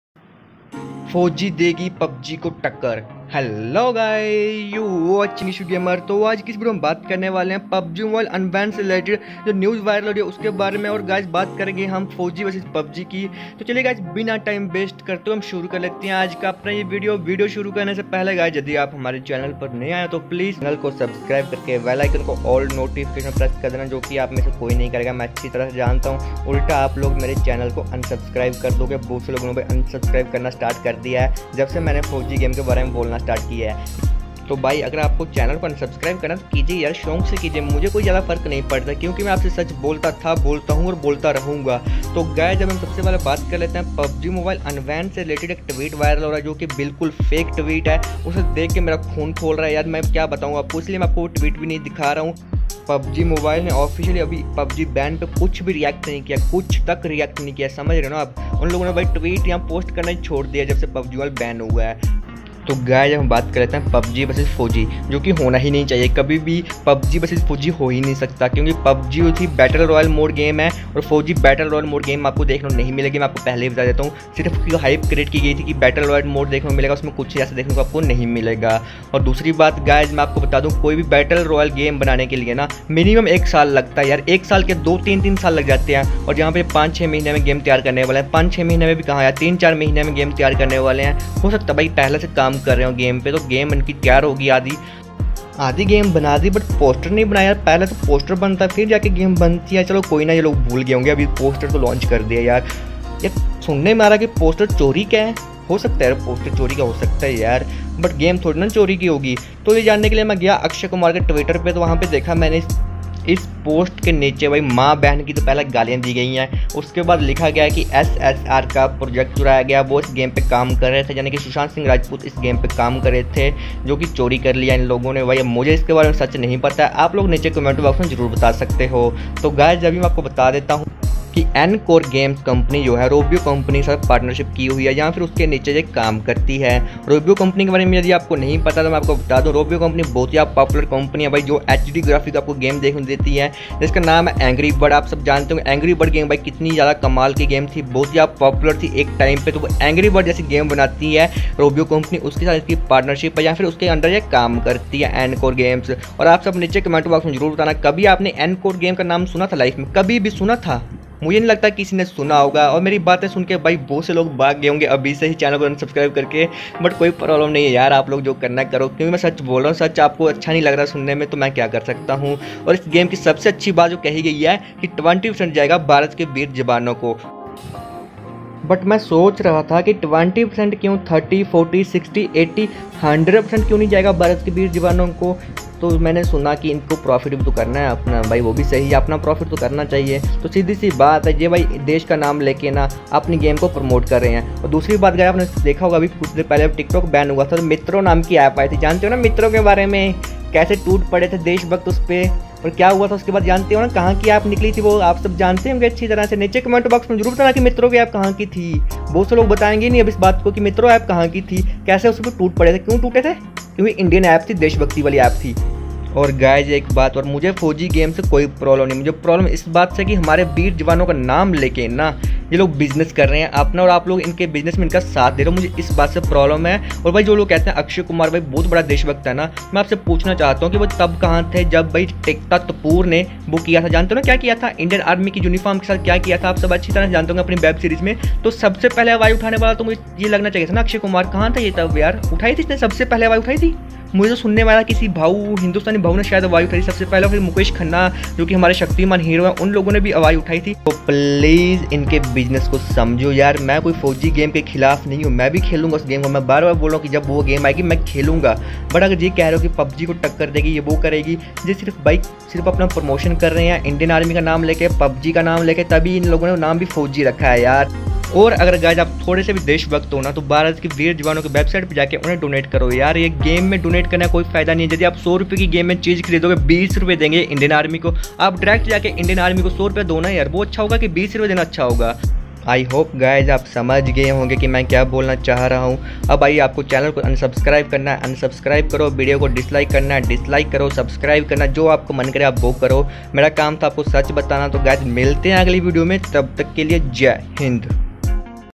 FAUG vs PUBG | PubgMobile unban New Update
Hello guyss iss video me pubg unban se related news  btayi hai aur faug vvs pubg 🤣😂
Donation (If u want to donate for help me to improve video quality and live stream then donate)

Pubg id Name~IshuGameryt

Thankyou♥️